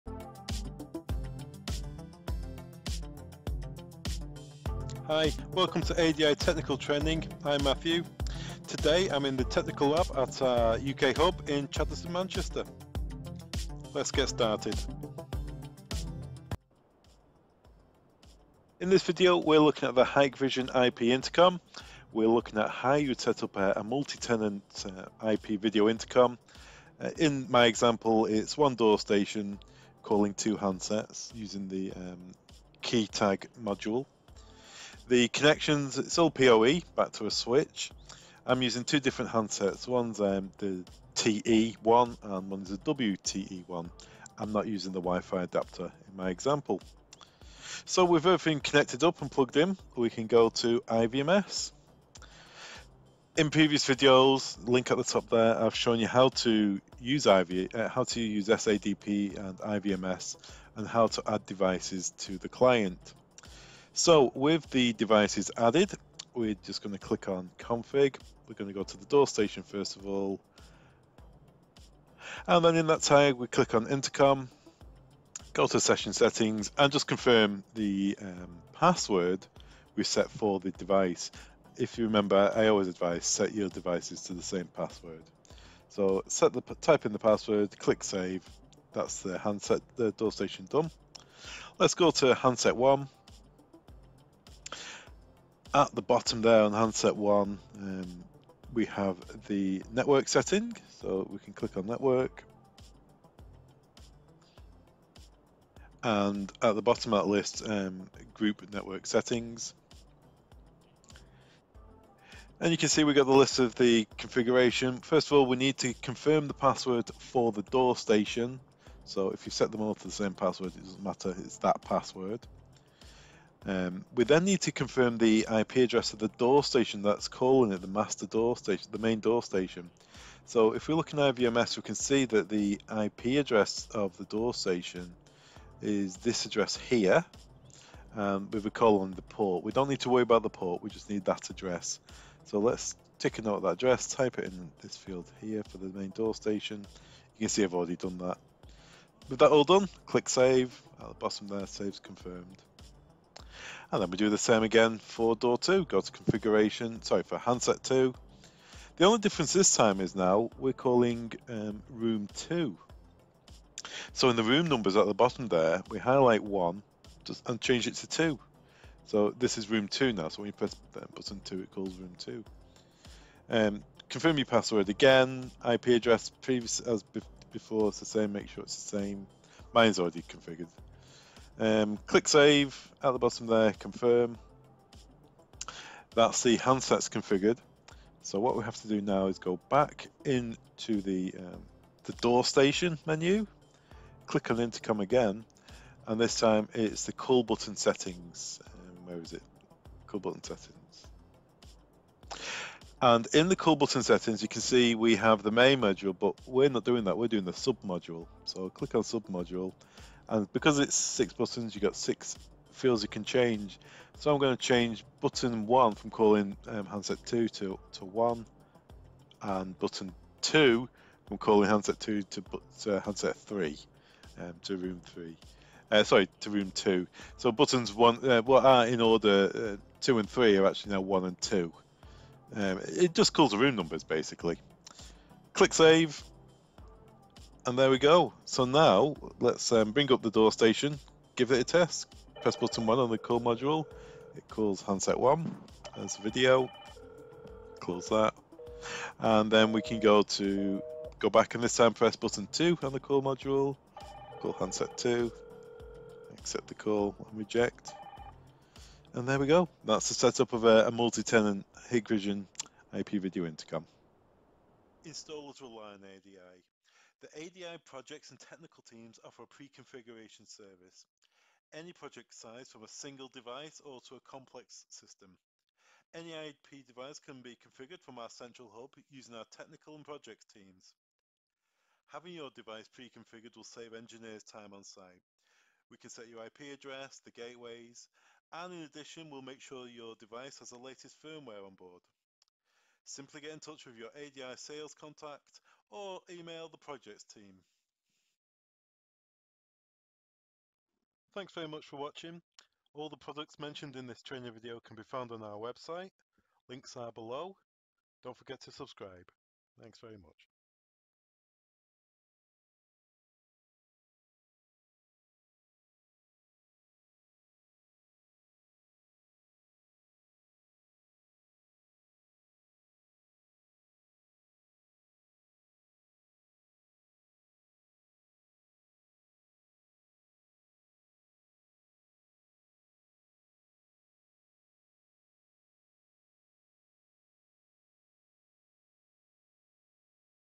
How to Setup a Multi-Call Hikvision IP Video Intercom DS-KH8350-TE1 and DS-KD-KK
Tutorial on how to set up the Hikvision IP video intercom with Hikvision monitors.